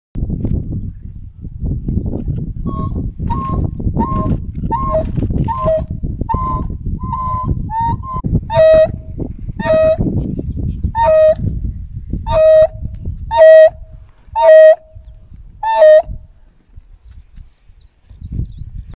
Wild Dog Call | Safari Snippets
African Wild Dogs call out for a lost member of their pack. A really special sighting, but they do sound like a broken wheelbarrow!